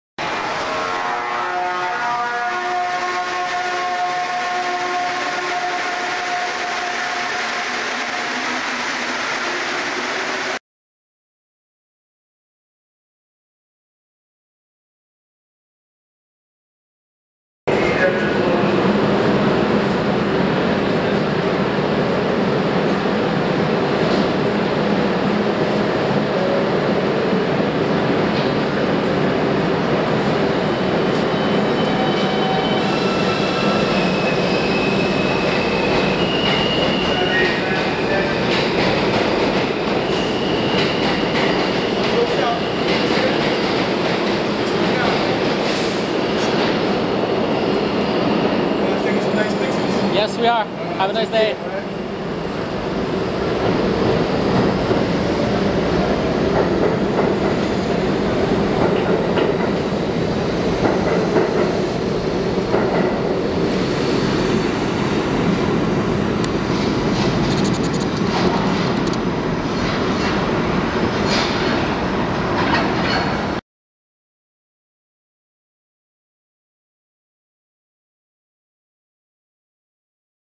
NYC Subway: West 4th Station [HD]
Here is a Video of West 4th Station of the New York City Subway in New York, New York.